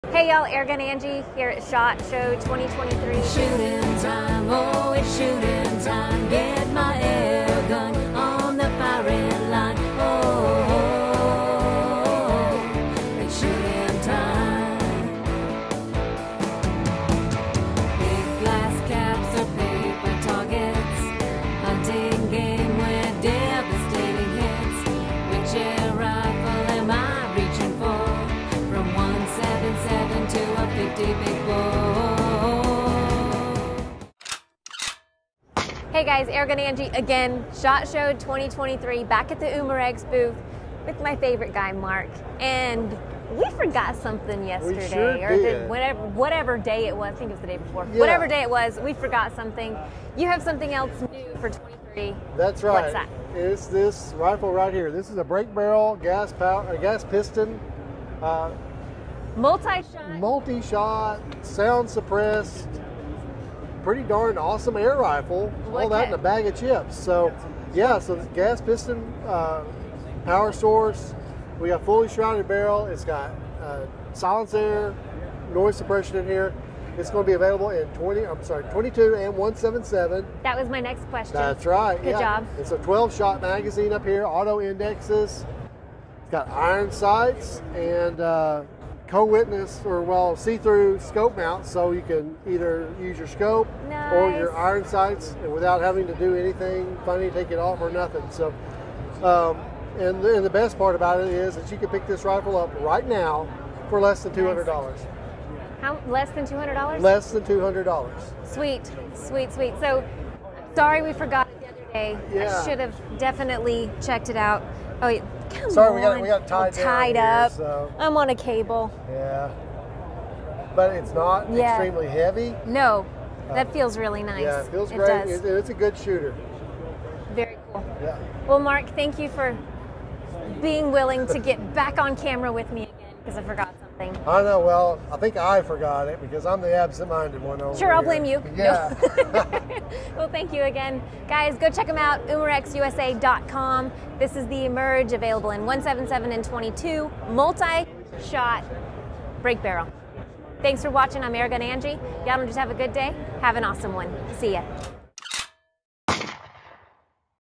SHOT SHOW ‘23 – UMAREX USA a Multi-Shot Break Barrel Too!
In this video we visit the Umarex USA booth at SHOT SHOW 2023, OH NO!! We left one out, the Umarex Emerge, it’s a multi-shot break barrel.